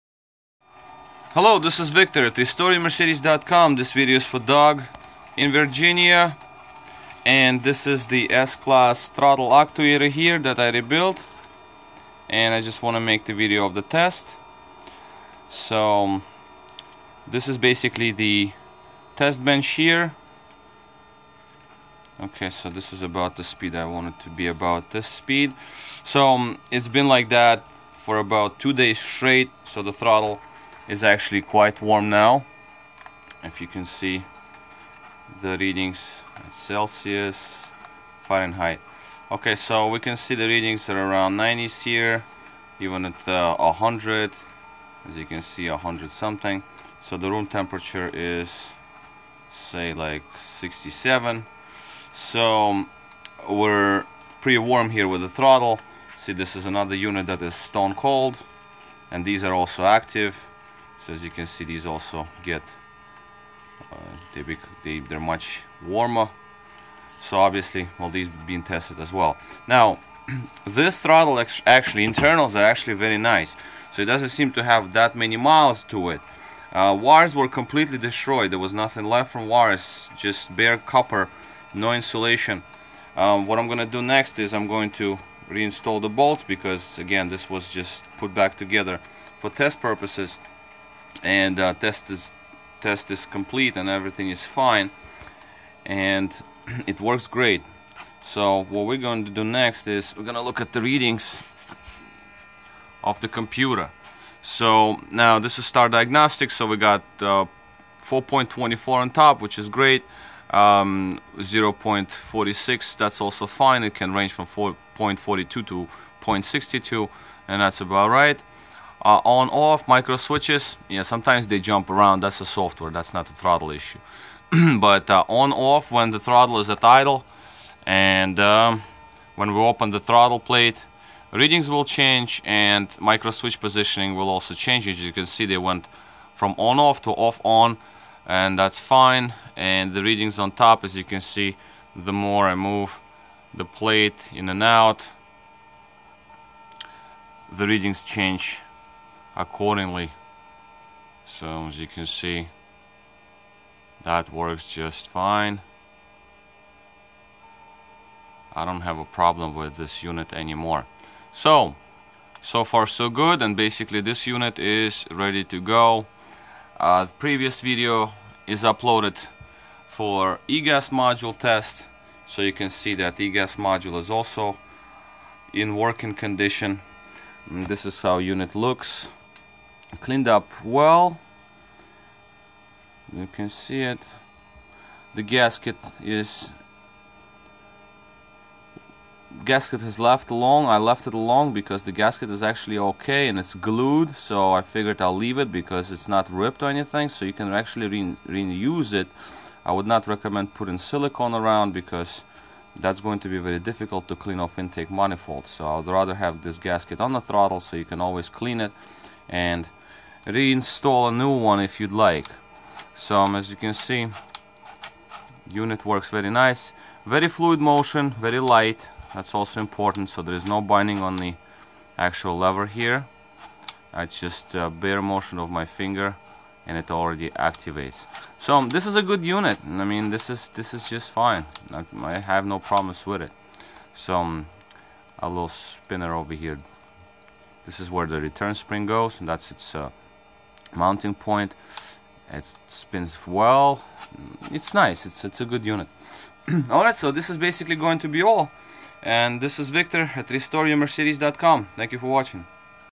Mercedes ASR throttle body repaired. Bench test
For Doug in VA
Repaired throttle actuator. Bench test

Independent Mercedes Benz diagnostic and repair service